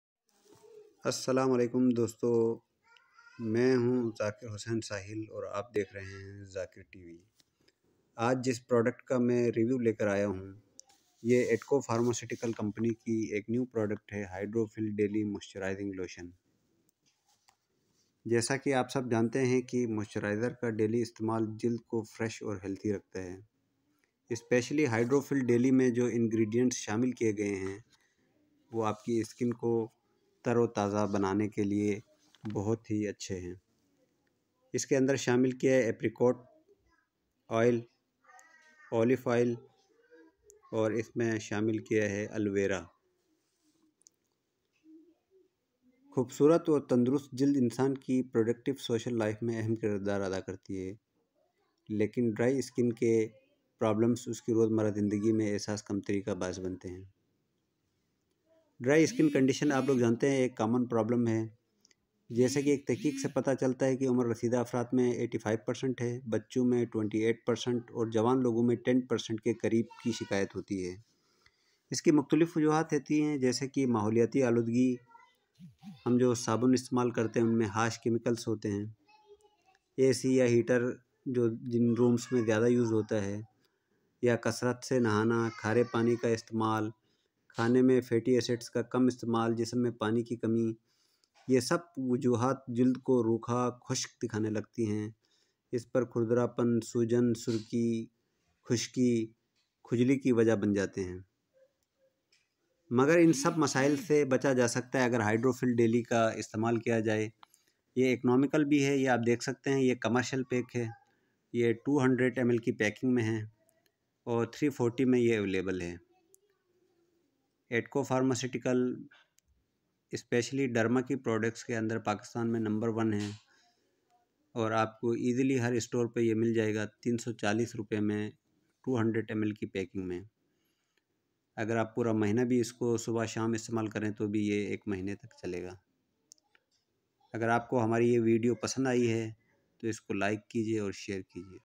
hydrophil Daily moisturizing lotion
for dry skin conditions
خشک جلد سے اب  ہمیشہ کے لئے نجات مل جائے گی ہائیڈرو فل ڈیلی روز مرہ استعمال کے لئے ہے اس میں شامل ہے الویرا اولیو آئل ایپریکوٹ ایل